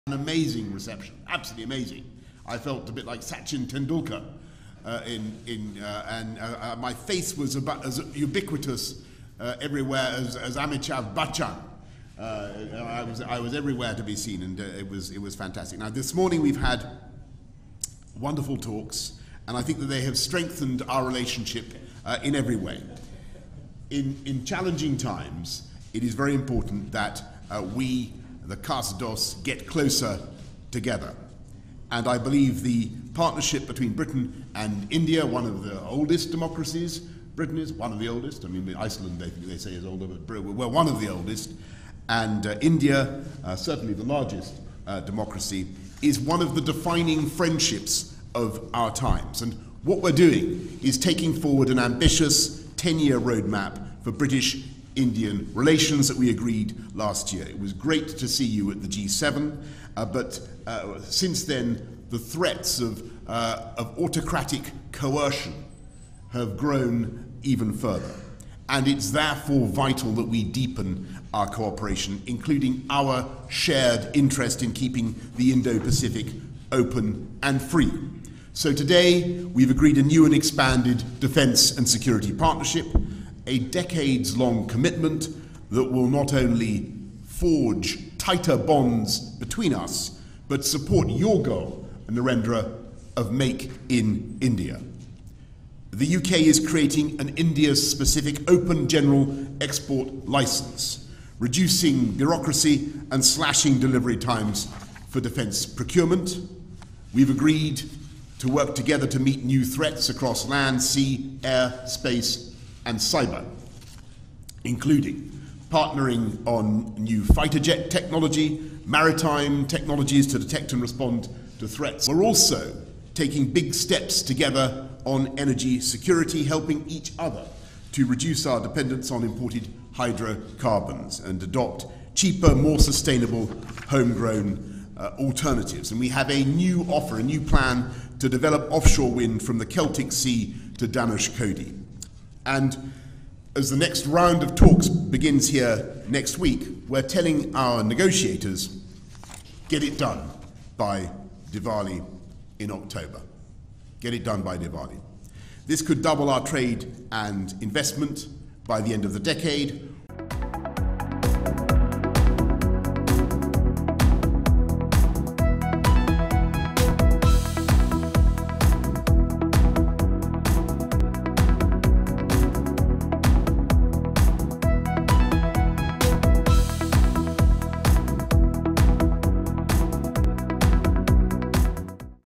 Boris Johnson :India के Style के दीवाने हुए , Sachin, Amitabh का नाम लेकर ये PM MODI से ये बोले
Download Tak App:
Boris Johnson :India के Style के दीवाने हए ब्रिटेन के PM, Sachin, Amitabh का नाम लेकर ये बोले